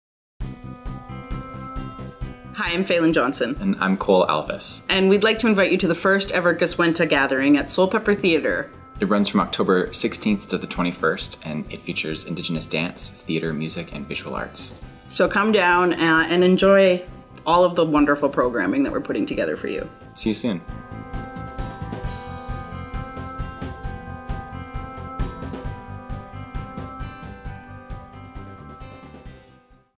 Guswenta Gathering
The Guswenta Wampum Belt, commonly referred to as the Two Row Wampum, is a mutual treaty agreement created in 1613 between the Haudenosaunee people and the Dutch settlers. The Belt ensures both parties conduct themselves in all proceedings as equals, contemporaries, each traveling in their own canoe without interrupting the other’s path.

Programmed by Falen Johnson and Cole Alvis, the Guswenta Gathering brings Indigenous artists to Soulpepper with presentations of theatre, dance, music and art. Programming will highlight art that speaks to the lands and waterways where The Young Centre for the Performing Arts sits.